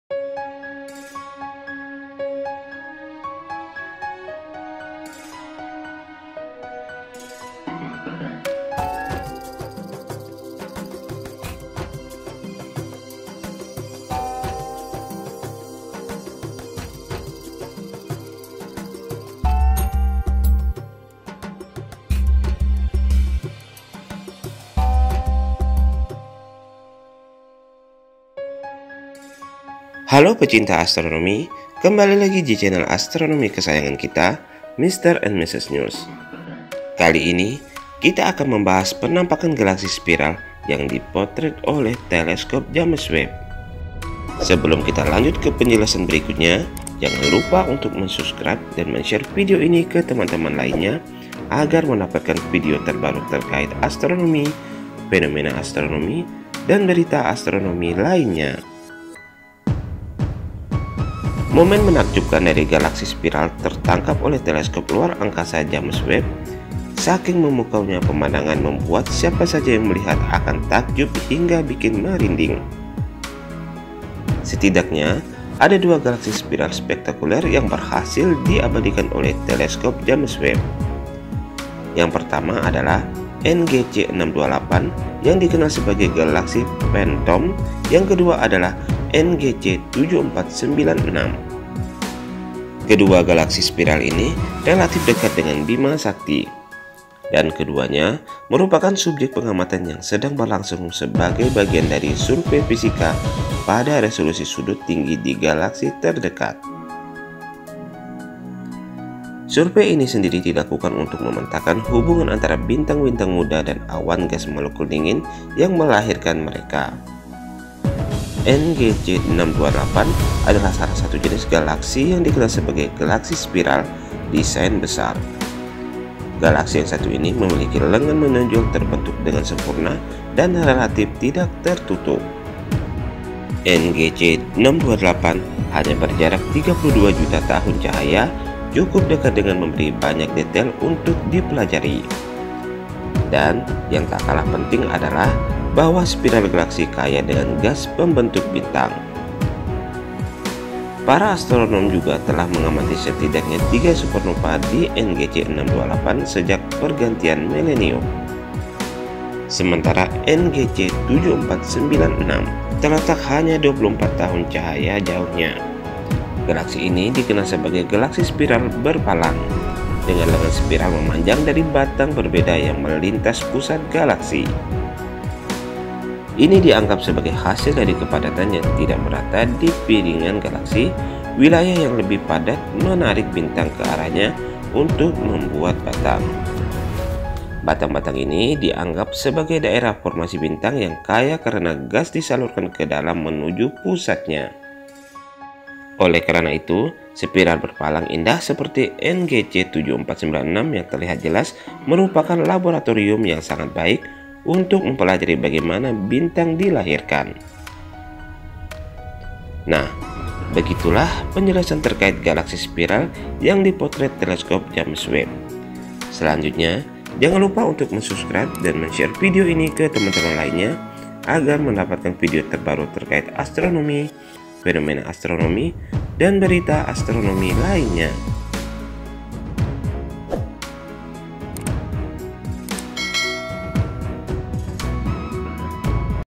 PENAMPAKAN GALAKSI SPIRAL DARI TELESKOP JAMES WEBB
TERIMAKASIH !!

Momen menakjubkan dari Galaksi Spiral tertangkap oleh teleskop luar angkasa James Webb (JWST). Saking memukaunya pemandangan, membuat siapa saja yang melihat akan takjub hingga bikin merinding. Setidaknya ada dua Galaksi Spiral spektakuler yang berhasil diabadikan oleh JWST. Yang pertama adalah NGC 628 yang dikenal sebagai Galaksi Phantom dan yang kedua adalah NGC 7496. Kedua Galaksi Spiral ini relatif dekat dengan Bima Sakti. Dan keduanya merupakan subjek pengamatan yang sedang berlangsung sebagai bagian dari survei Fisika pada resolusi Sudut Tinggi di Galaksi Terdekat (PHANGS). Seperti dilansir dari Science Alert, Sabtu (23/7), survei ini sendiri dilakukan untuk memetakan hubungan antara bintang-bintang muda dan awan gas molekul dingin yang melahirkan mereka. NGC 628 adalah salah satu jenis galaksi yang dikenal sebagai Galaksi Spiral “desain besar”. Galaksi yang satu ini memiliki lengan menonjol, terbentuk dengan sempurna, dan relatif tidak tertutup.
NGC 628 hanya berjarak 32 juta tahun cahaya, cukup dekat untuk memberi banyak detail untuk dipelajari. Dan yang tak kalah penting adalah bahwa spiral galaksi kaya dengan gas pembentuk bintang. Para astronom juga telah mengamati setidaknya tiga supernova di NGC 628 sejak pergantian milenium. Sementara NGC 7496 terletak hanya 24 juta tahun cahaya jauhnya. Galaksi ini dikenal sebagai Galaksi Spiral berpalang, dengan lengan spiral memanjang dari batang berbeda yang melintasi pusat galaksi. Ini dianggap sebagai hasil dari kepadatan yang tidak merata di piringan galaksi, wilayah yang lebih padat menarik bintang ke arahnya untuk membuat batang. Batang-batang ini dianggap sebagai daerah formasi bintang yang kaya karena gas disalurkan ke dalam menuju pusatnya.